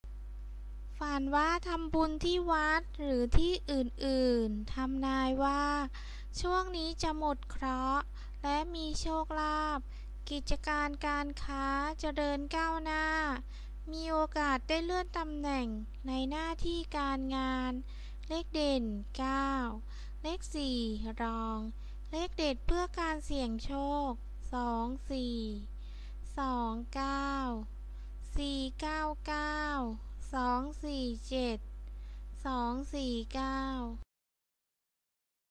ฝันว่าทำบุญ
ฝันว่าทำบุญ จะเป็นที่วัด หรือทำบุญที่อื่นๆ ทำนายว่าจะหมดเคราะห์ และมีโชคลาภ